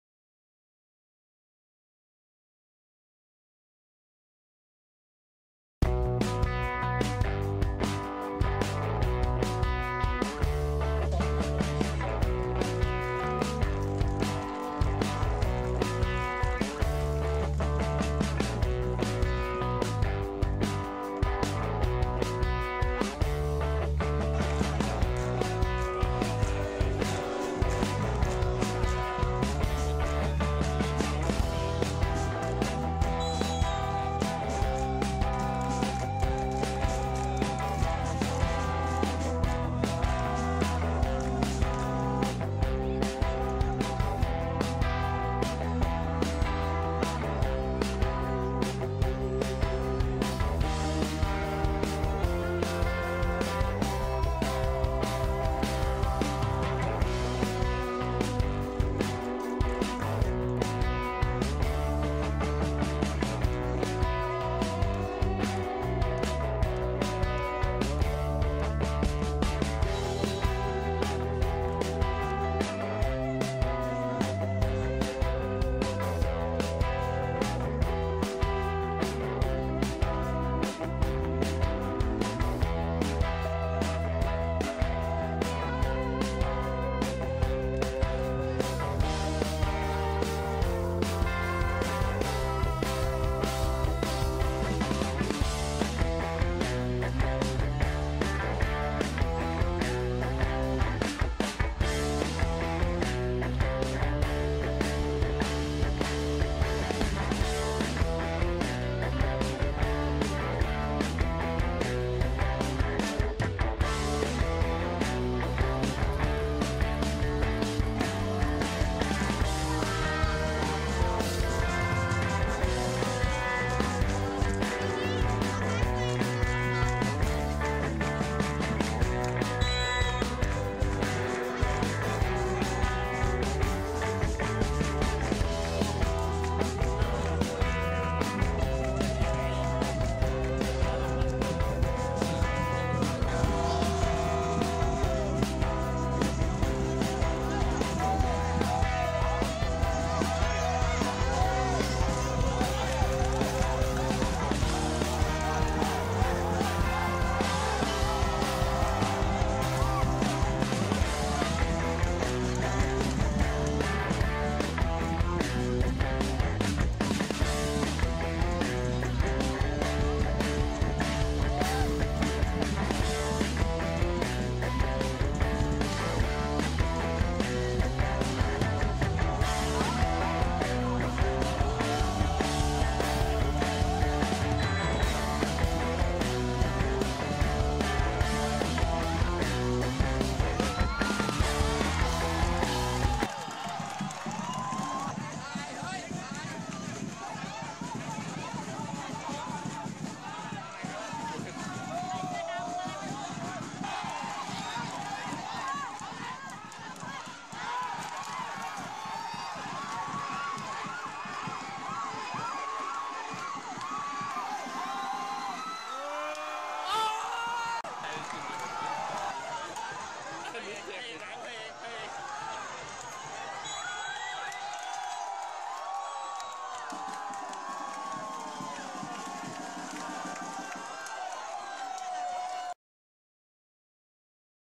HIGHLIGHT : PHAYAO FC 0-1 PHREA UTD
EURO CAKE THAI LEAGUE 3: 2017 UPPER Region
MATCH8 on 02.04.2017 at Phayao Provincial Administrative Organization Stadium (PAO stadium), Phayao Province